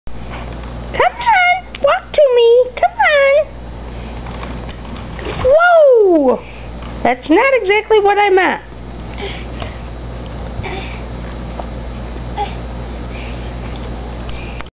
skyler falling under walker
while climbing on the outside of the walker he falls under it